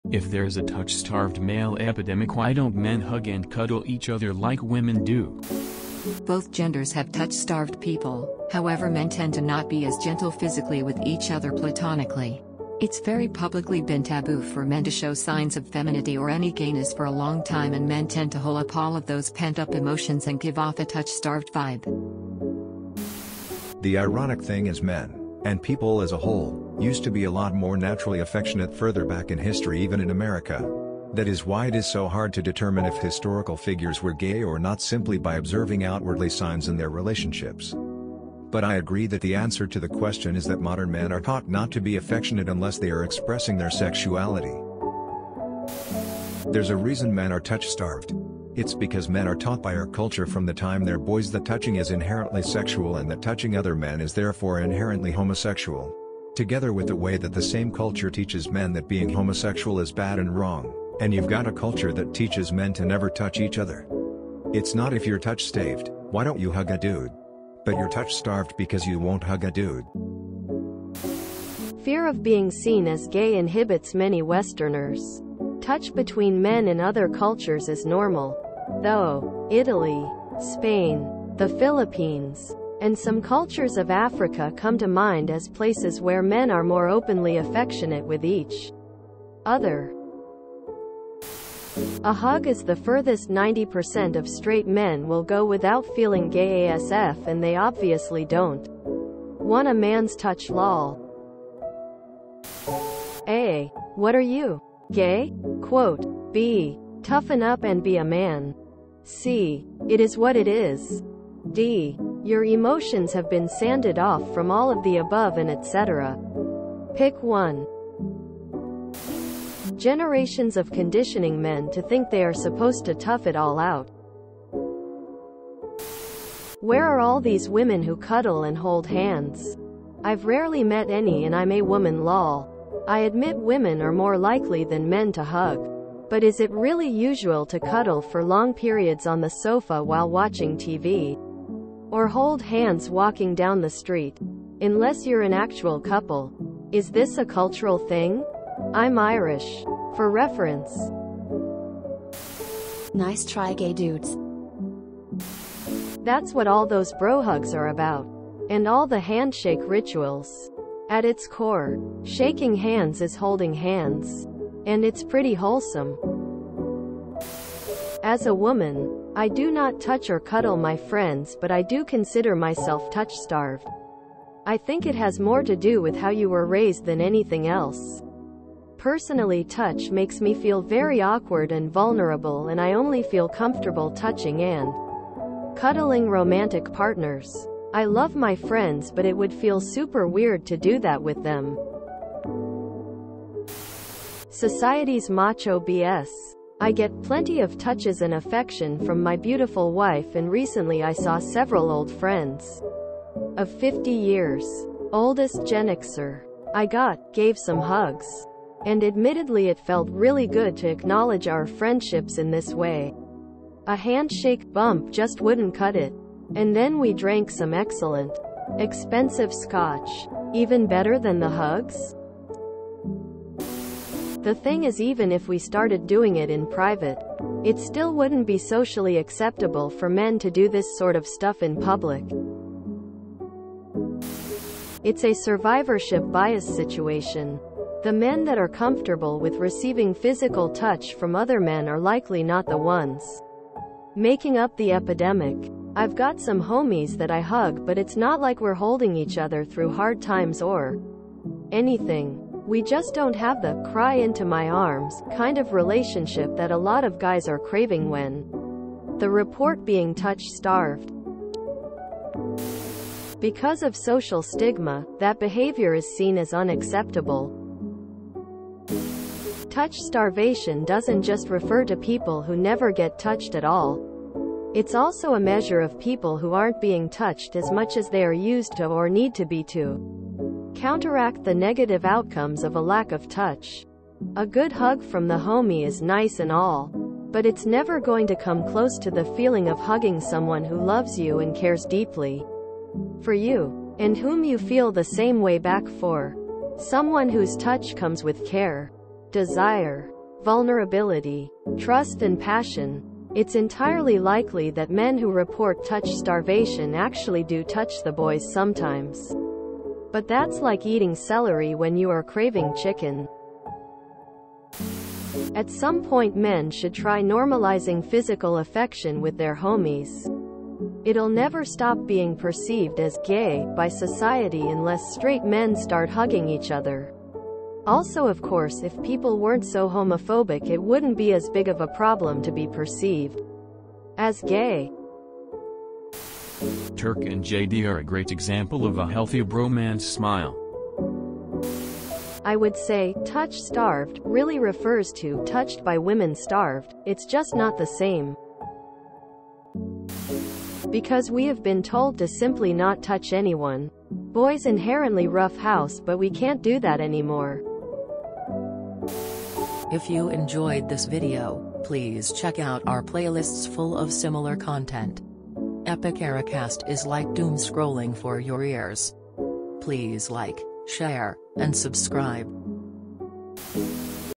If there is a "touch-starved male epidemic" why don't men hug and cuddle each other like women do?
If there is a "touch-starved male epidemic" why don't men hug and cuddle each other like women do? (r/TooAfraidToAsk)

Source: redleaves939 on Reddit (link removed at the insistence of YouTube)